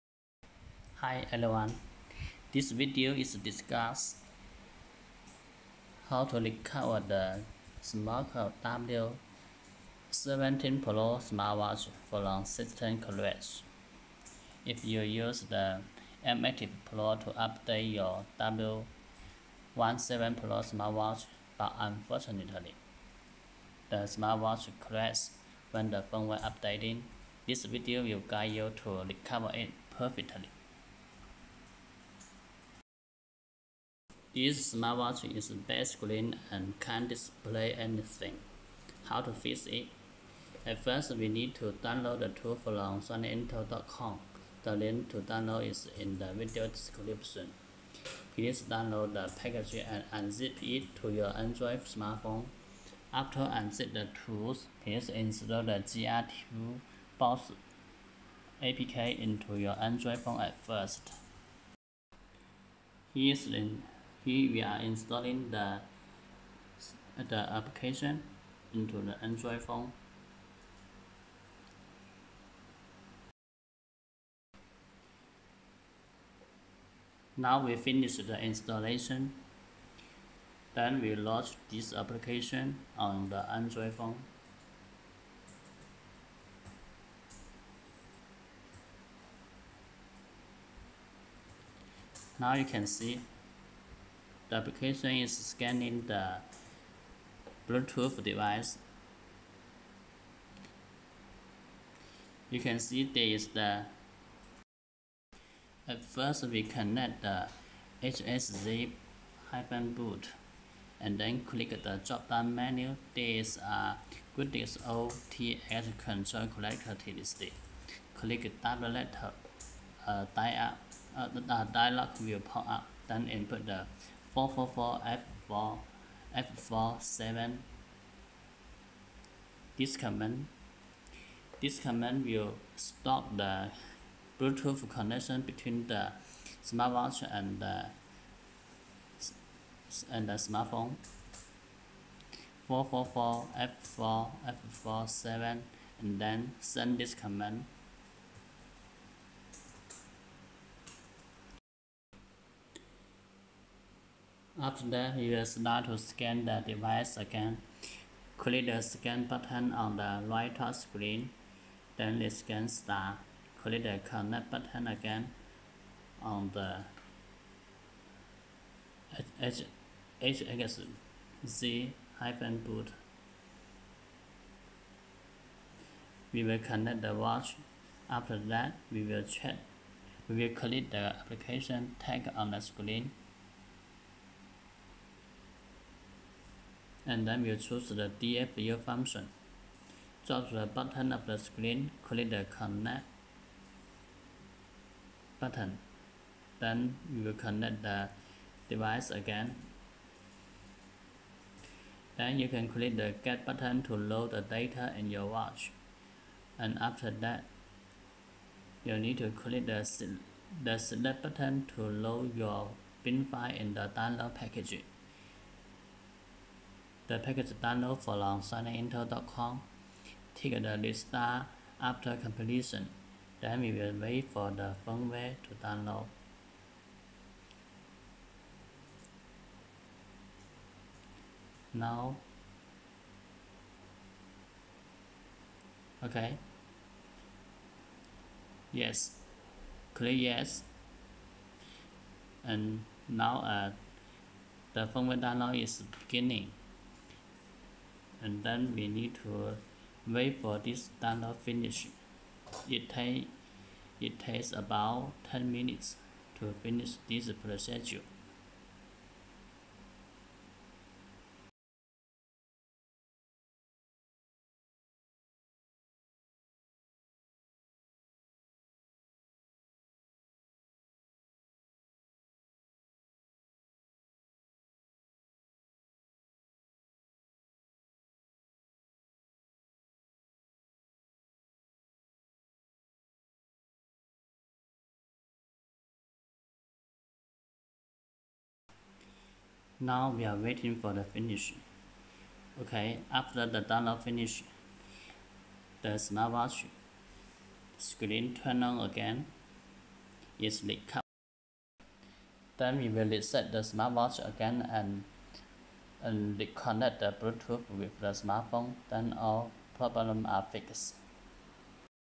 how to recover a system crashed smartwatch: smochm w17 pro smart watch recover
Smochm is a powerful smartwatch supplier worldwide, this video information are from shiningintl.com, the smartwatch professional supplier. I will demonstrate how to recover the smochm w17 pro smart watch from system crash. if you use the Mactive pro to update your w17 pro smartwatch, but unfortunetely, the smart watch crashed when the firmware updating, this video will guide you to recove it perfectly.

you see that this smartwatch is black screen and can't display anything, how to fix it? at first ,we need to download the tools from shiningintl.com, the link to download is in the video description. please download this package and unzip it to your android smart phone. after unzip the tools, please install the GRToolbox-v2.03.apk into your android phone at first.

at first we connect the HXZ-BOOT, and then click the dropdown menu, there is a "goodix OTX control chracteristic", click the "W" letter, a dialog will pop up, then input the "444F4F47".

after that, we will start to scan the devices again, click the "scan" button on the right-top screen, then rescan, click the "connect" button again, we will connect to the watch . after that, we will click the "application" tag on the screen, and then choose the "DFU" function, drop to the bottom of the screen, click the "connect" button, then connect the "HXZ-BOOT", then you can click the "get" button to load the data in your watch.   and click the "select" button to load your bin file in the download package, download from shiningintl.com, tick the "restart after completion", then we will wait for the firmware download, it takes about 10 minutes to finish.

after the download finish, the smart watch succeed to recover, then reset the watch  in setting  and reconncet to smart phone, all work normally now!